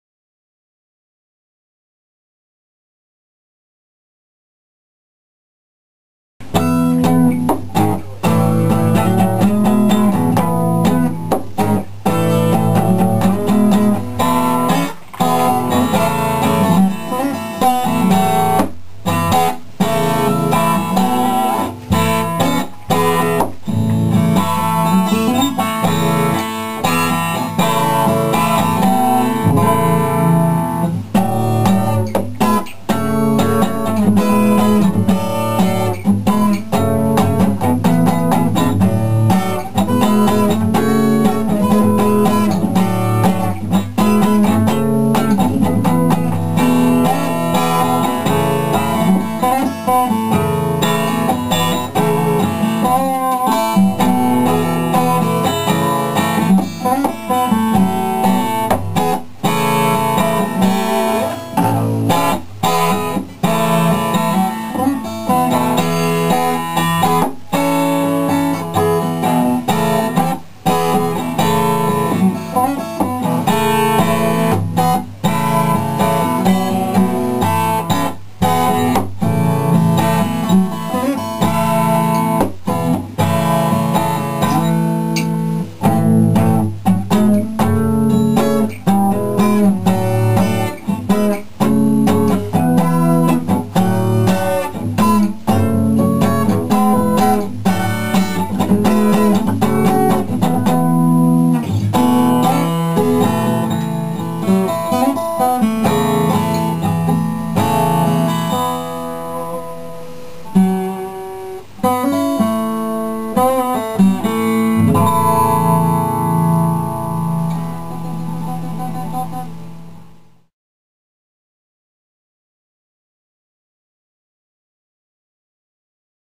Guitar)
Me playing guitar') Dont criticize too much. I'm just playing the melody,I like. And play quite bad ;)